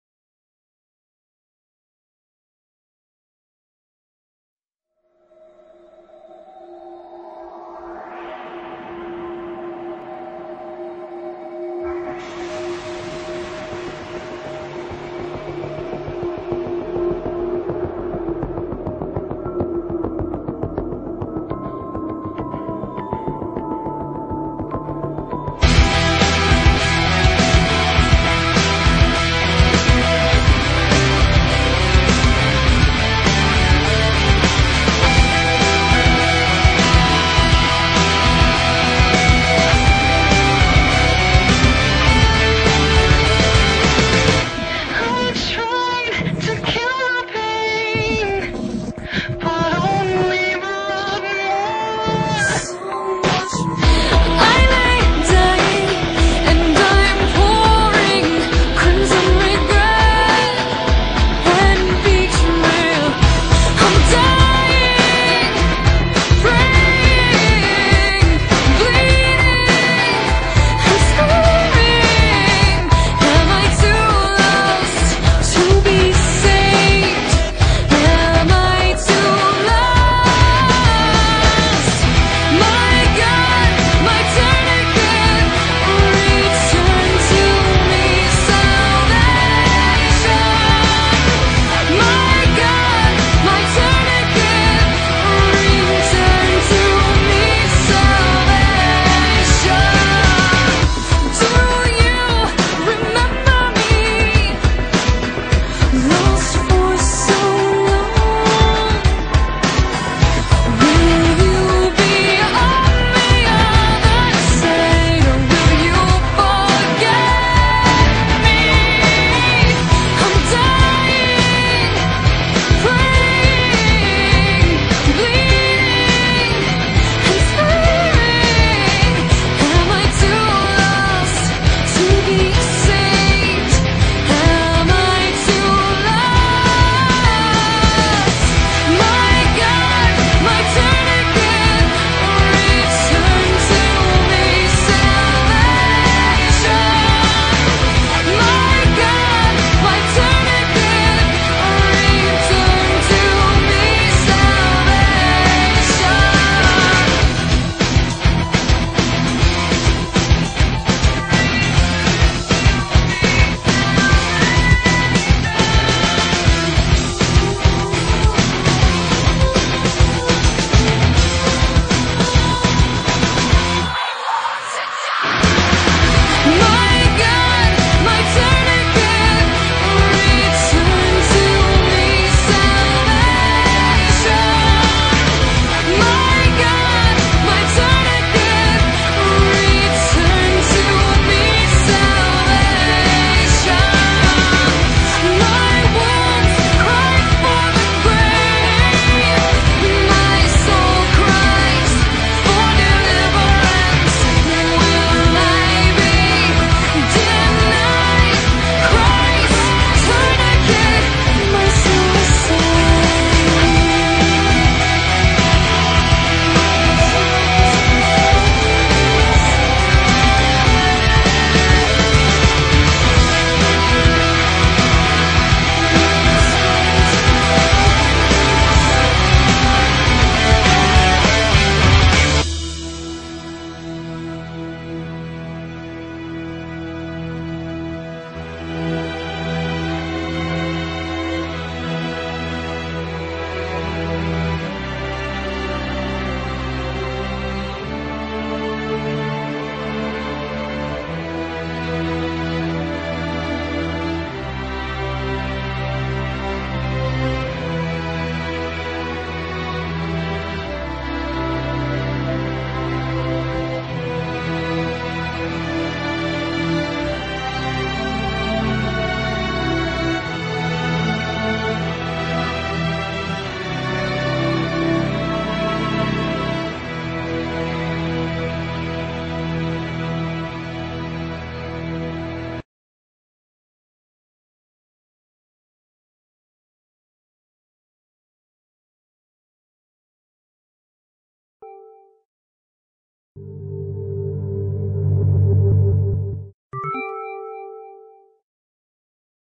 the coolest queens of rock
this is a slideshow of anette and amy lee
they are so cool and good to sing